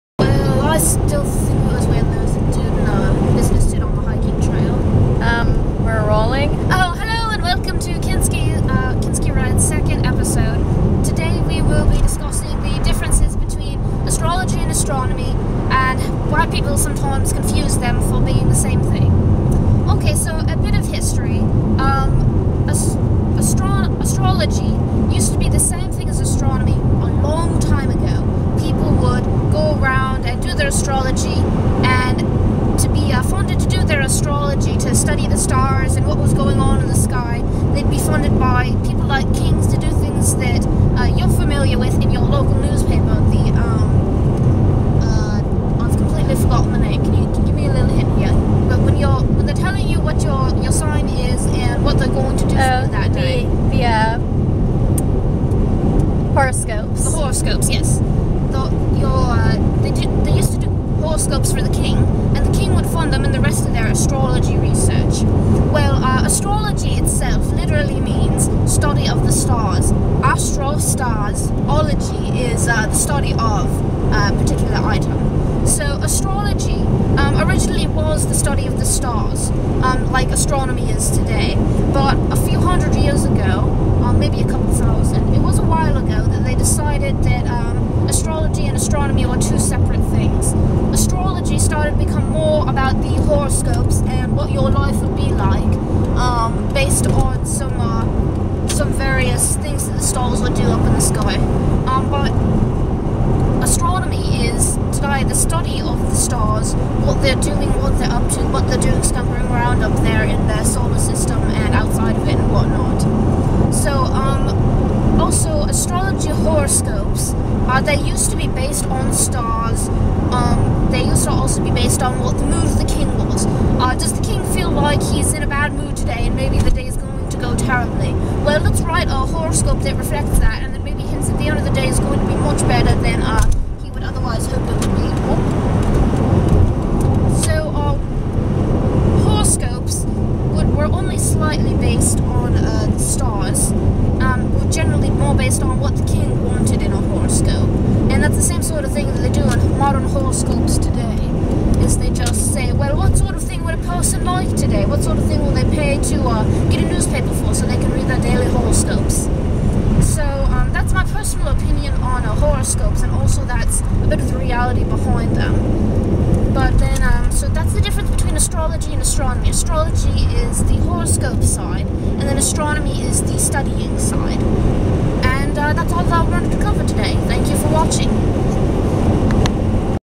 002 - Astrology Vs Astronomy
In which I discuss astrology and astronomy, the differences between the two, and the history of astrology.